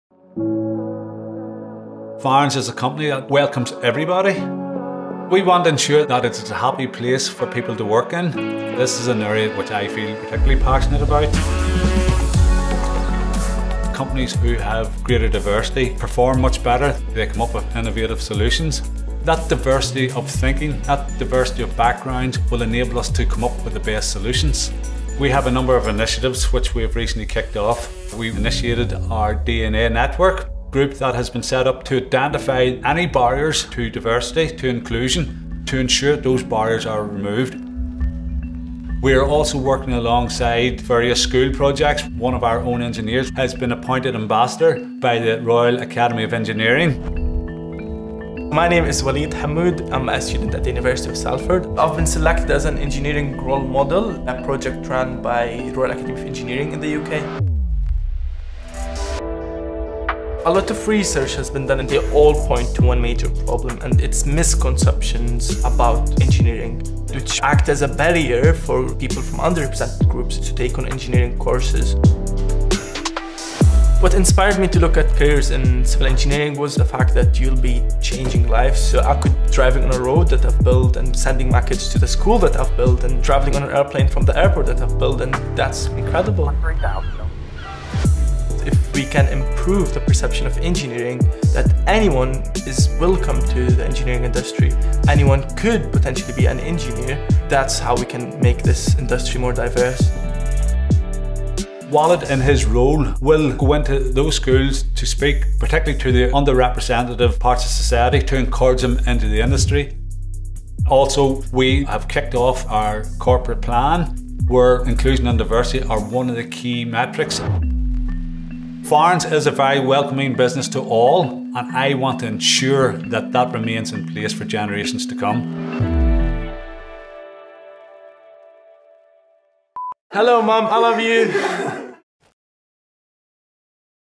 Diversity at Farrans
At Farrans we know that having a blend of experiences, ideas and perspectives makes us a stronger and more innovative company.

We are taking action to make sure that the construction and civil engineering industry is inclusive for all for generations to come.

Find out more about our work in this important area.